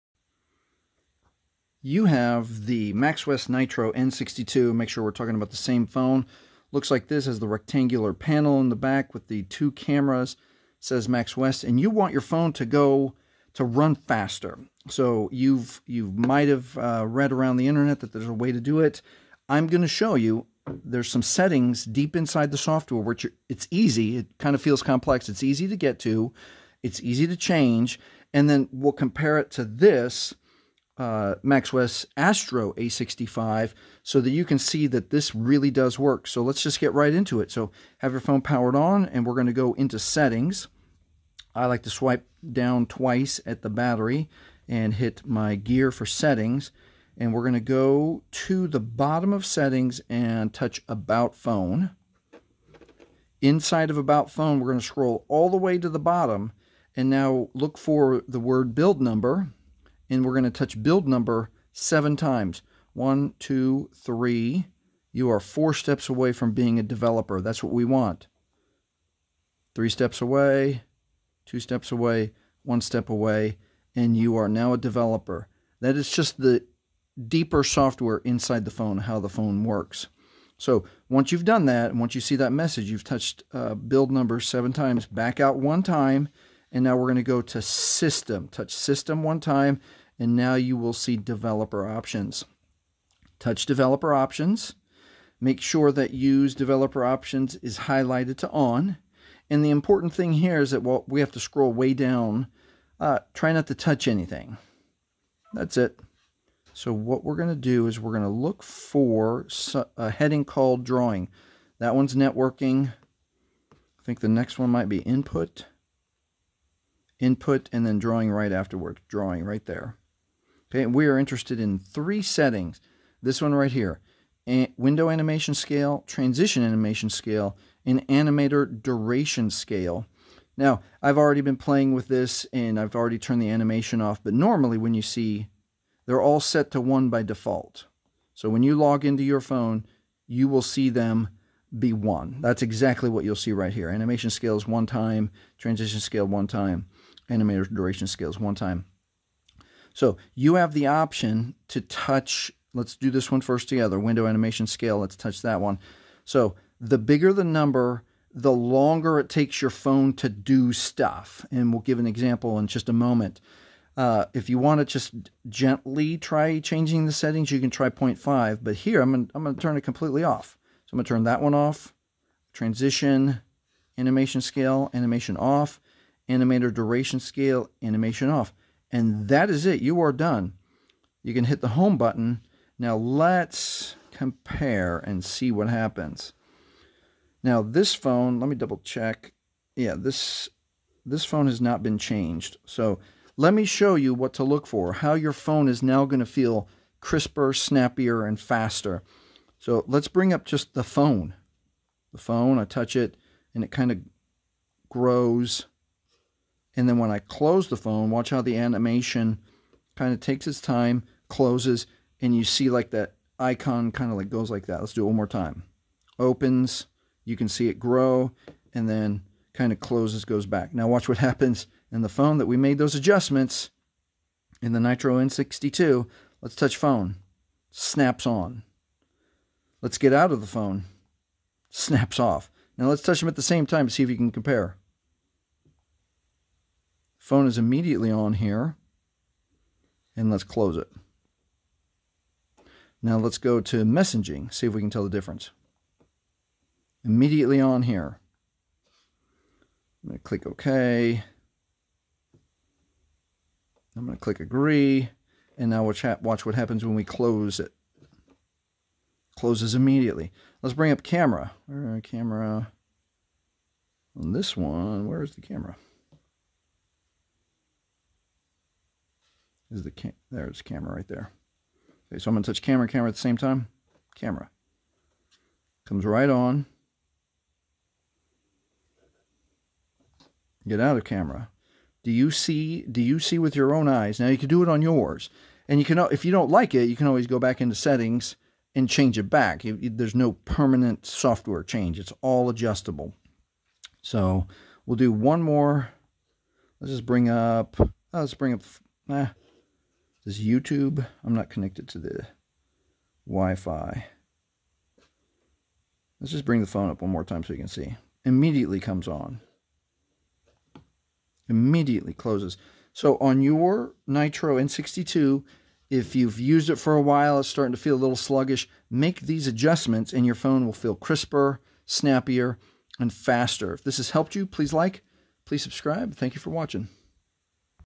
Maxwest Nitro N62 - Make My Maxwest Phone Run Faster - Tested - This Really Works!
This really works. Watch this video to see how to easily change a few settings inside of developer mode (IT IS SUPER EASY!!!) and you can see - with real examples in this video how much faster this Maxwest Nitro N62 runs.

This impressed me! It doesn't double the speed of your phone, but it does make it go faster!!!

Change THESE Settings to Make Your Maxwest Nitro N62 Run Faster

Hey, IS YOUR PHONE CASE UGLY??